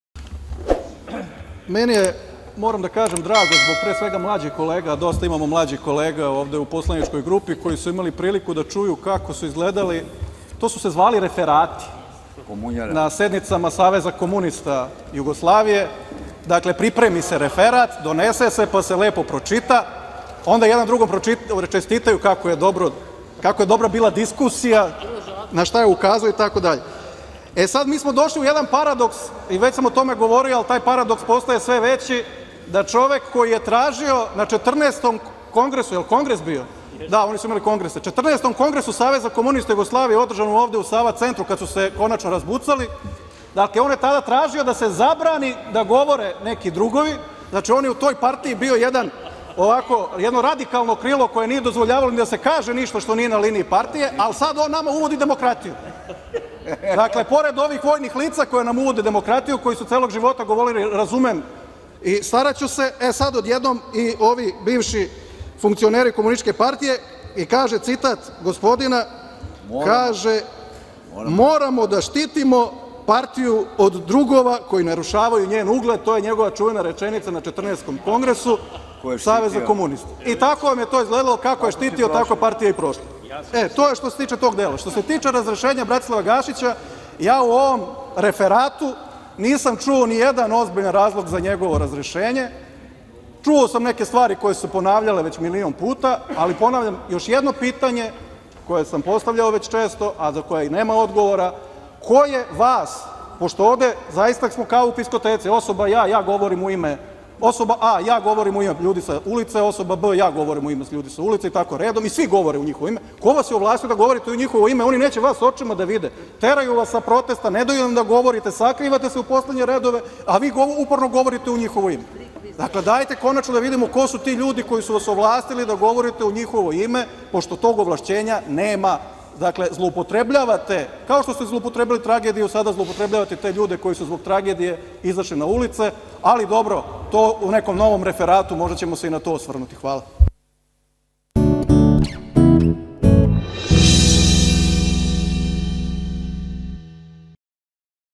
Скупштина Србије⎜🤦🏻‍♂️ Док се држао Вебера било је смисла, а онда је КРЕНУО СВОЈЕ И СВЕ УПРОПАСТИО
Одговор посланику Зорану Лутовцу, ДС
наставак Трећег ванредног заседања НCPC у Тринаестом сазиву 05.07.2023.